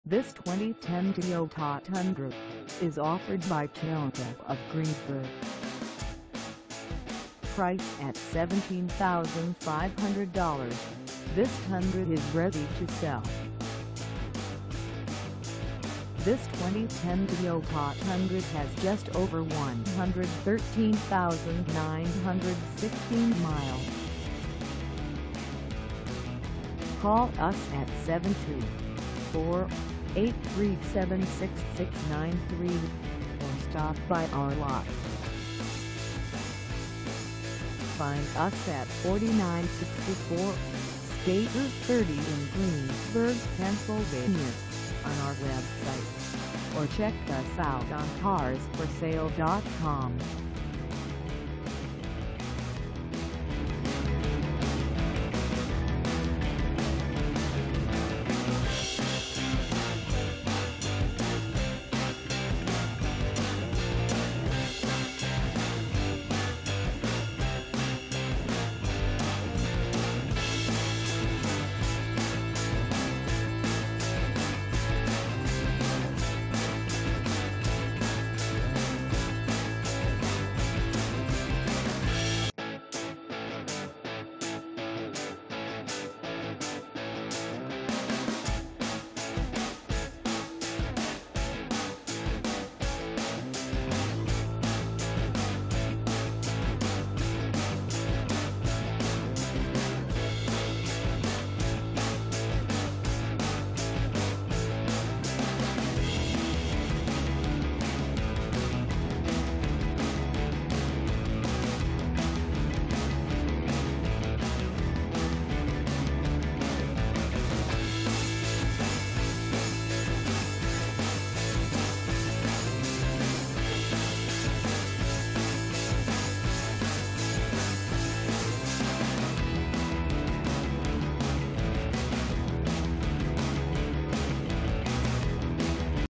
2010 Toyota Tundra Grade 4x4 4dr Double Cab Pickup SB (5.7L
This 2010 Toyota Tundra Grade 4x4 4dr Double Cab Pickup SB (5.7L V8) is for sale in Greensburg, PA 15601 at TOYOTA OF GREENSBURG.

Engine: 5.7L V8
Transmission: Automatic 6-Speed
Color: Pyrite Mica
Mileage: 113916
Contact TOYOTA OF GREENSBURG 4964 STATE ROUTE 30 Greensburg, PA 15601 to test drive this 2010 Toyota Tundra Grade 4x4 4dr Double Cab Pickup SB (5.7L V8) today.